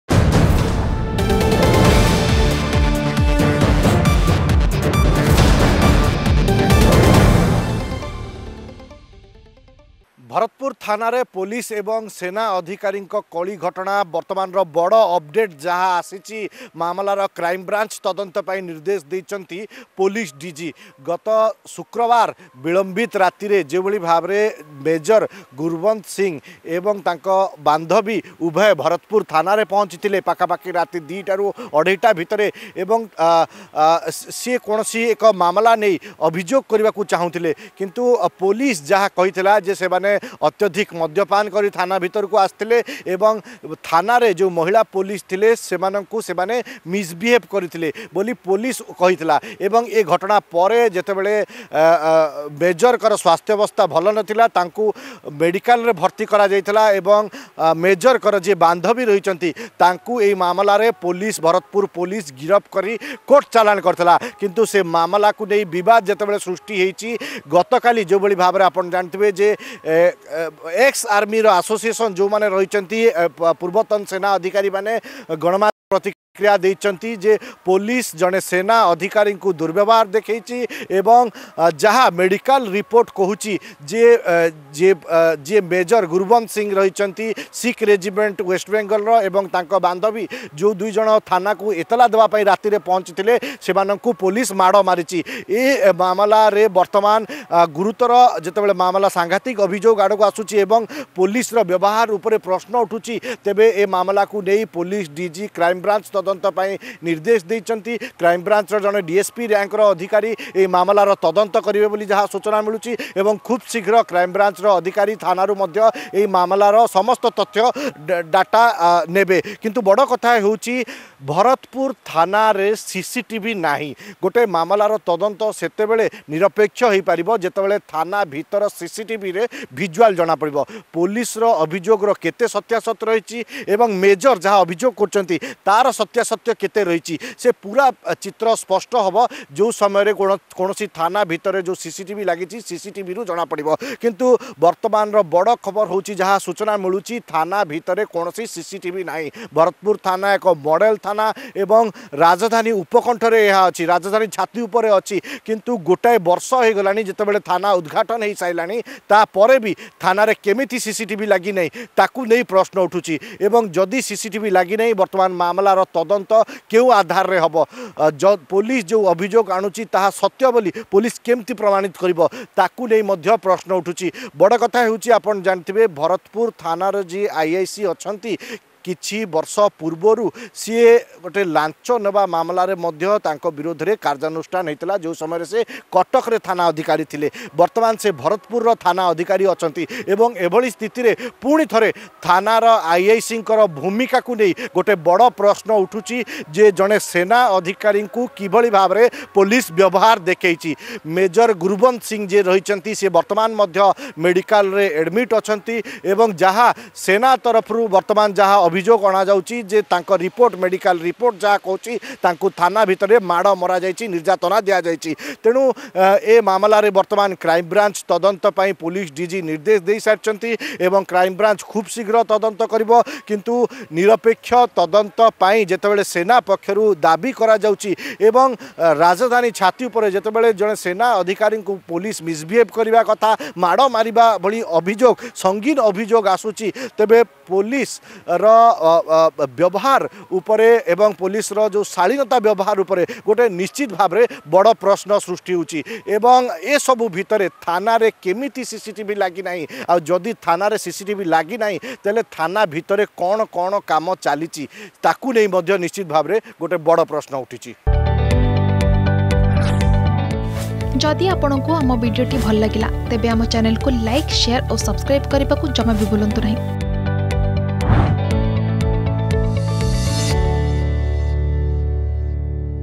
ଭରତପୁର ଥାନାରେ ସେନା ପୋଲିସ କଳି ଘଟଣା || Bhubaneswar Crime Branch
ଭରତପୁର ଥାନାରେ ସେନା ପୋଲିସ କଳି ଘଟଣା । ଘଟଣାର ତଦନ୍ତ କ୍ରାଇମବ୍ରାଞ୍ଚ କରିବାକୁ ପୋଲିସ ଡିଜିଙ୍କ ନିର୍ଦେଶ ।